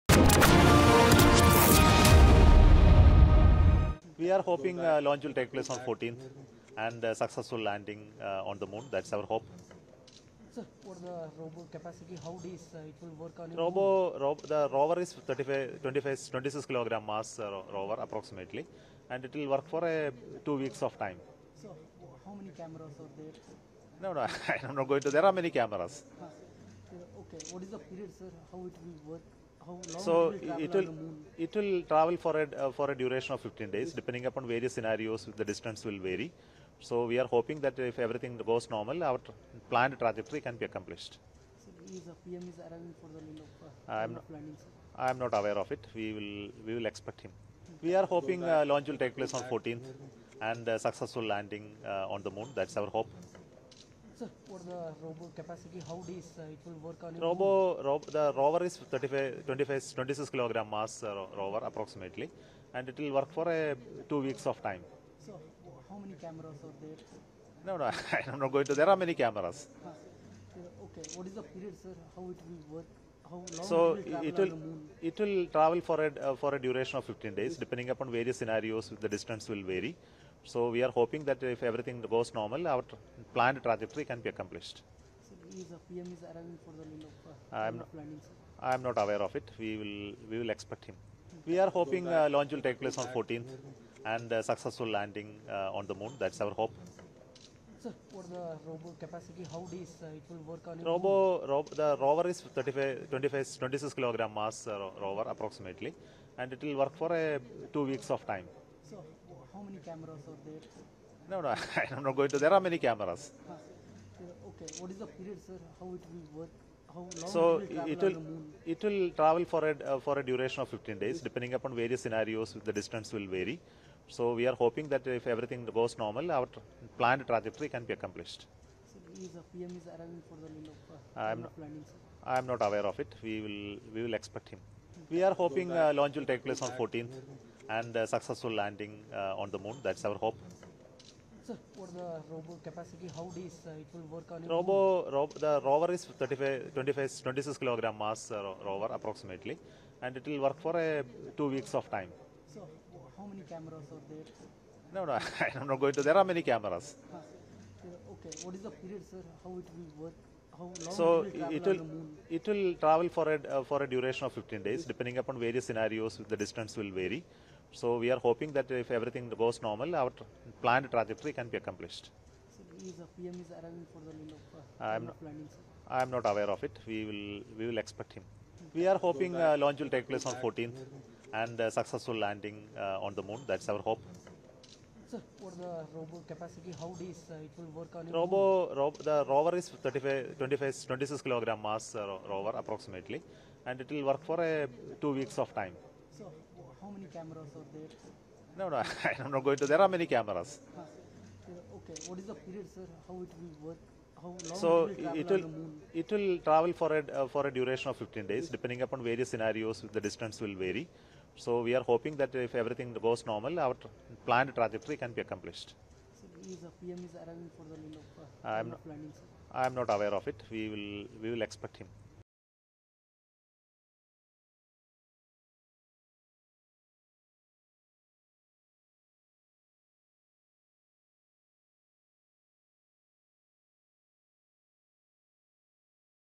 Chandrayan 3 लॉन्‍च पर बोले ISRO के अध्यक्ष S. Somanath,'सफलतापूर्वक चांद पर उतरेगा चंद्रयान 3'
चंद्रयान 3 पर भारतीय अंतरिक्ष अनुसंधान संगठन (ISRO) के अध्यक्ष एस. सोमनाथ बोले क‍ि हम 14 (जुलाई) को (अंतरिक्ष यान) लॉन्च करेंगे और उम्मीद करते हैं कि वह सफलतापूर्वक चांद पर उतरेगा. रोवर 26 किलो वजनी है जो 2 हफ्ते तक (चांद पर) काम करेगा. रोवर में काफी कैमरे लगे हैं.